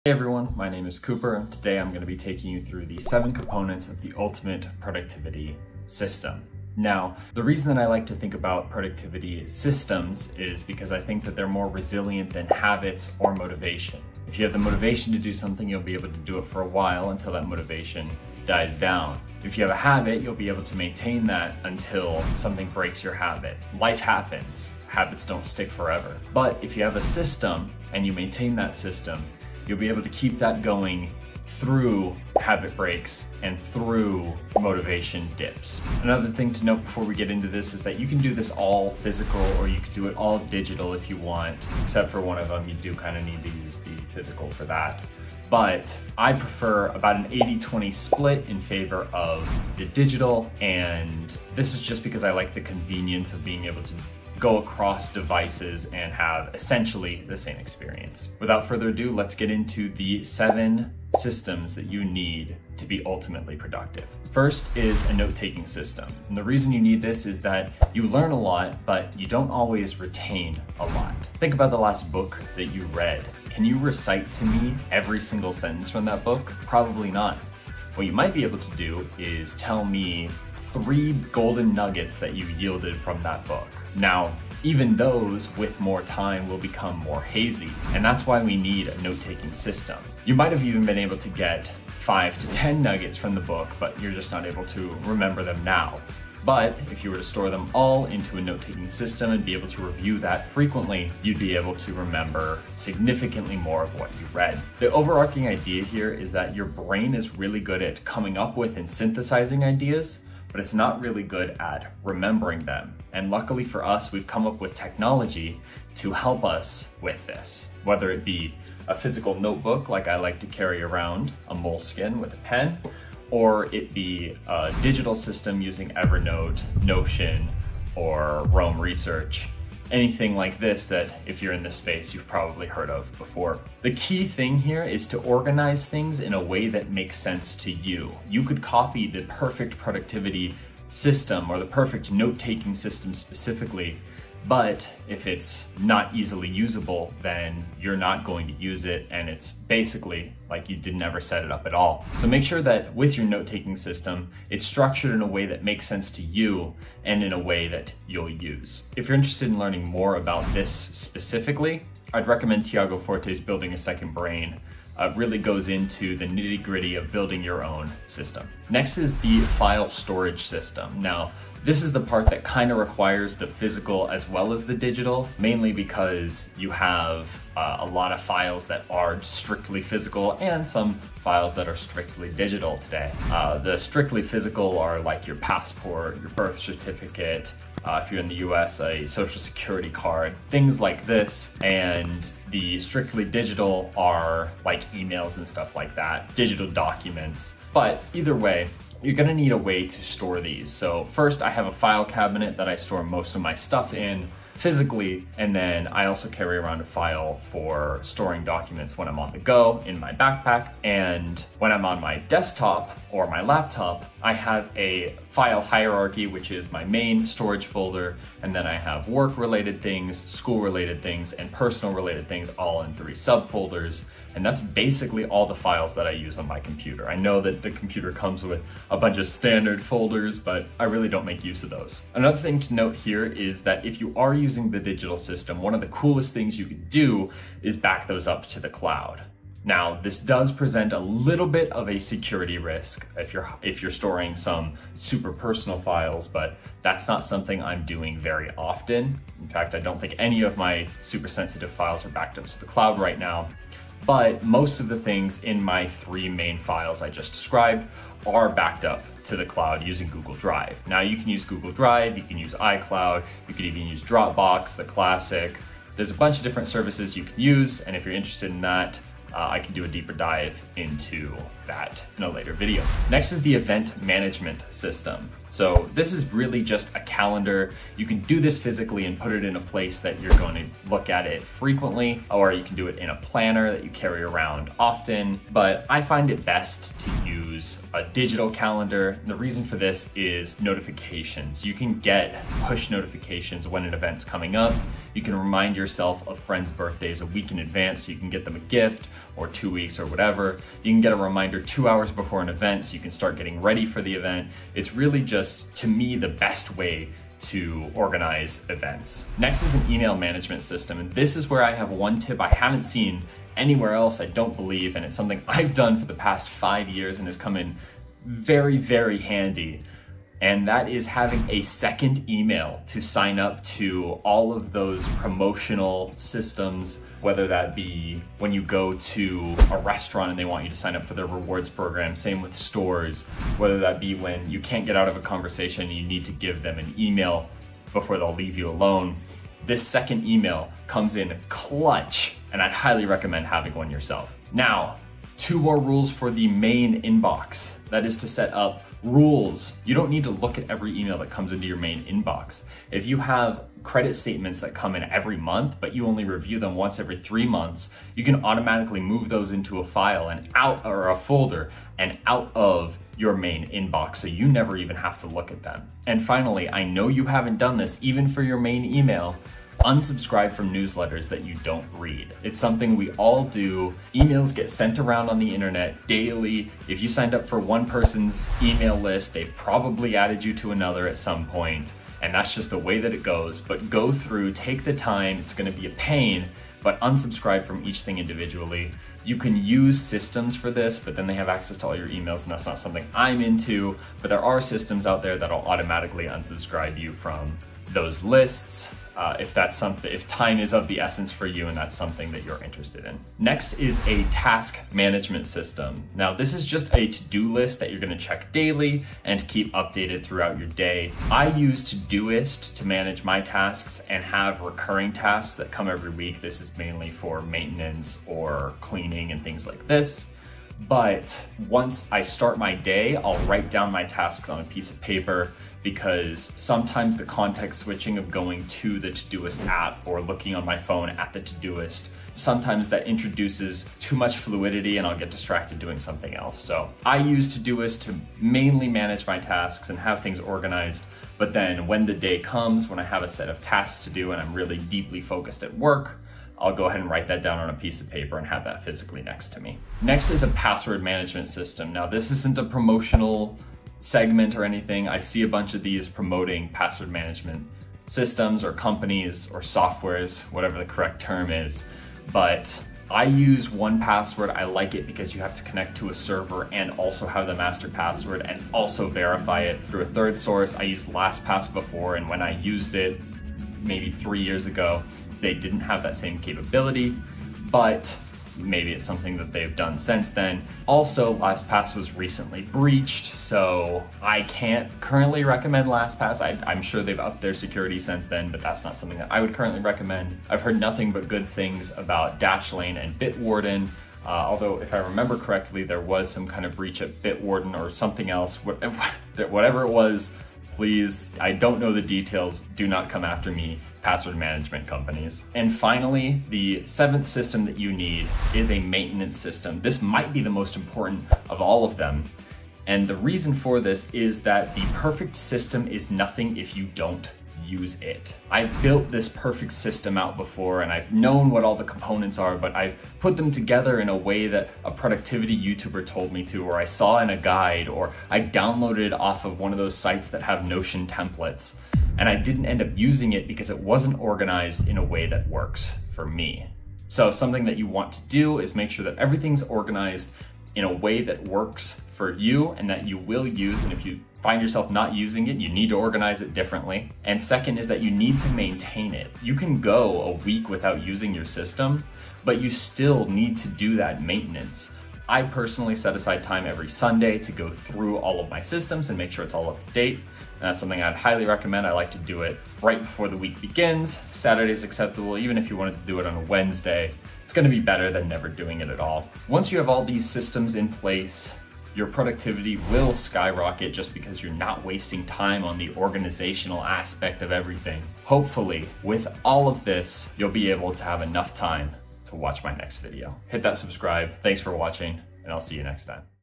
Get Organized in 2024 (7 Systems You Need)
Staying organized is essential to getting things done. In this video I cover the 7 components of the ultimate productivity system.

Please let me know what you think of the editing and video quality! I'm looking at upgrading my videos so all feedback is helpful!